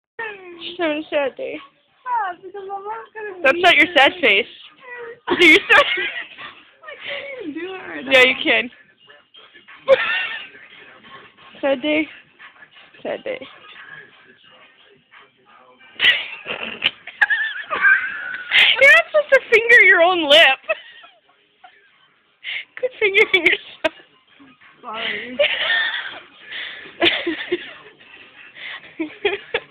You're NOT supposed to finger your own lip!
Nik was about to leave me and go back to PISSBURGH and she was sad she wasn't going to be with me for the holidays and this was one of our last nights in the garage. She was having a sad day (so was I) and she was pouty and high, this is what happens. Love those days always. At least I have the memories of us before she turned back into her PA self and she pretty much slipped out of my life. Wish it didn't end up like that but, things can always change right? Miss you Nik.